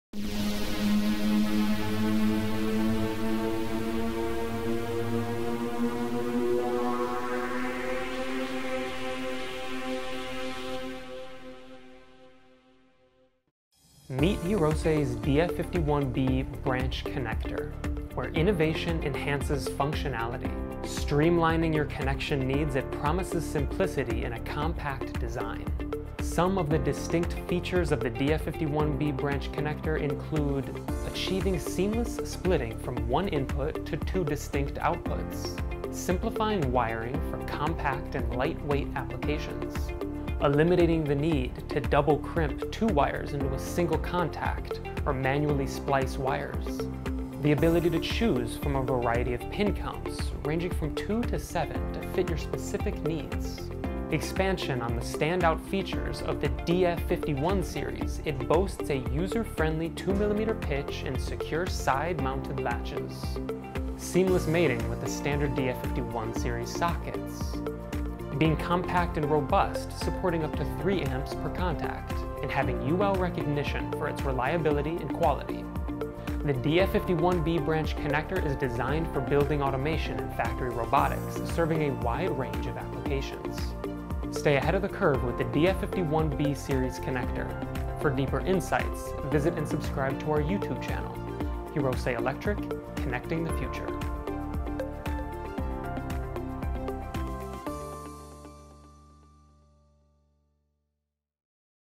DF51B | Positive Lock 2mm Pitch Wire-to-Board Connector Branch Adapter | Hirose Electric
Meet Hirose’s DF51B Branch Connector: where innovation enhances functionality.

FEATURES:
• Achieve seamless splitting from one input to two distinct outputs.
• Eliminate the need to "double crimp" two wires into a single contact or manually splice wires.
• Choose from a variety of pin counts, ranging from 2 to 7, to fit your specific needs.
• Expanding on the standout features of the WF51 series, it boasts a user-friendly 2mm pitch and secure side-mounted latches.
• Seamlessly mates with the standard DF51 series sockets.
• Compact and robust, supporting up to 3A per contact."
• UL Recognized for its reliability and quality.

APPLICATIONS:
Tailor-made for building automation and factory robotics, the DF51B Branch Connector is also versatile enough for applications in automotive electronics, smart devices, and medical technology.